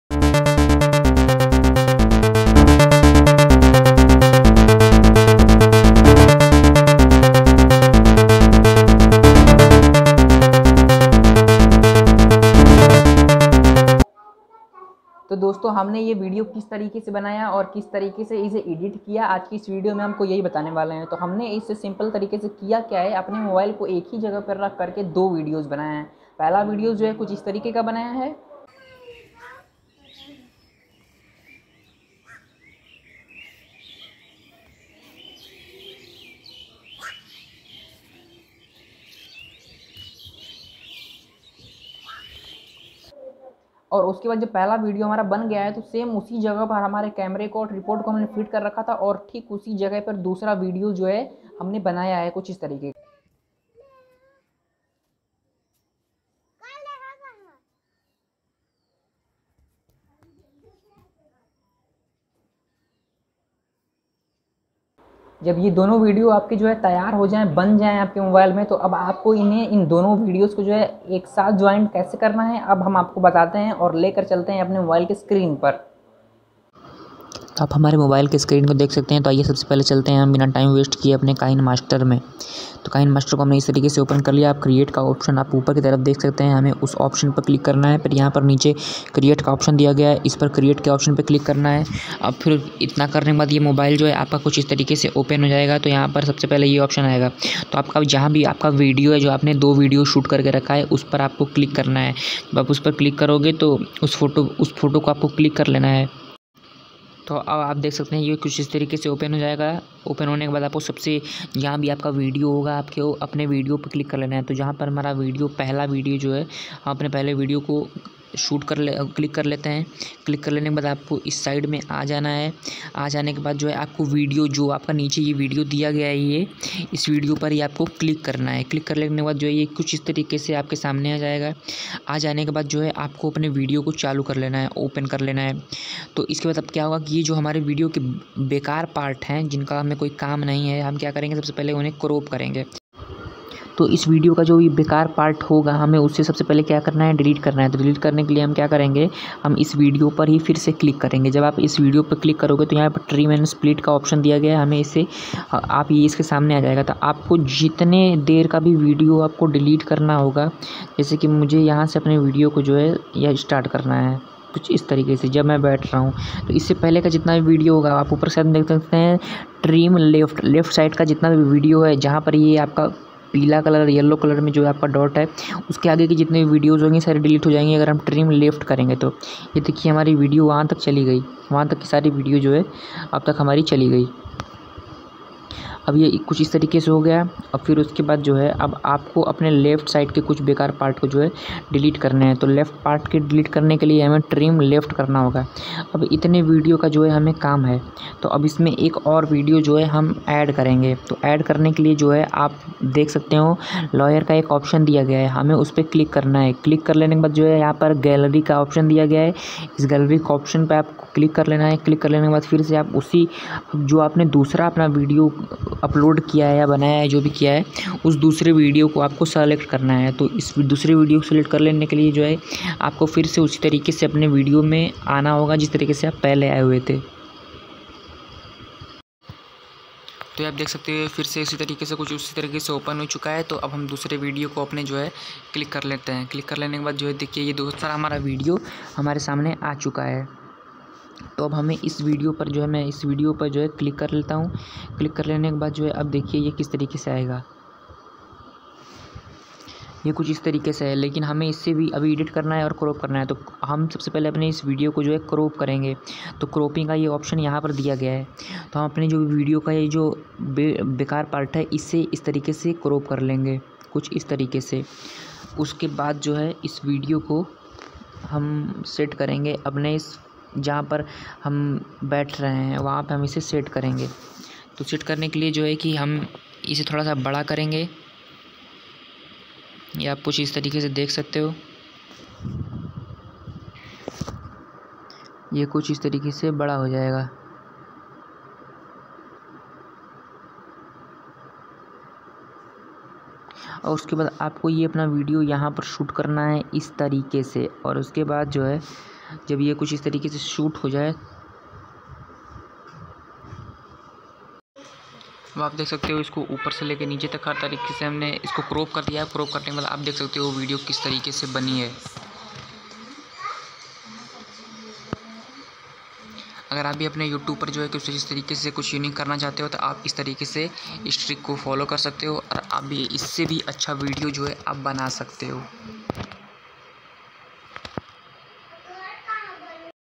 How to make double roll video in kinemaster||double roll video editing
dosto is video me mene kinemaster se video editing karna btaya hai or ye sikaya hai ki aap kese apne phone se double roll video editing kar sakte ho_